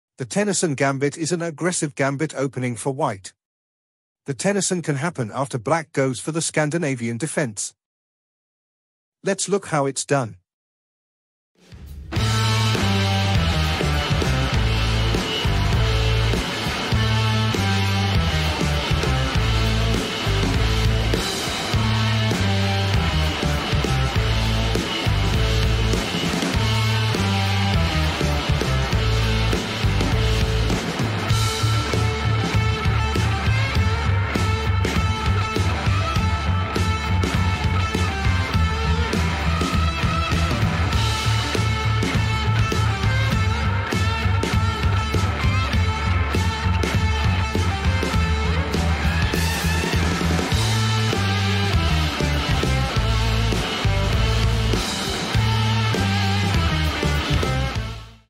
Ballistic Missile 🚀🚀 Gambit
Explore the strategic brilliance of the Tennison Gambit, also known as the ICBM (Intercontinental Ballistic Missile) Strategy, in this thrilling chess short. Discover how this aggressive opening unleashes a barrage of tactical firepower, leaving opponents reeling and defenses shattered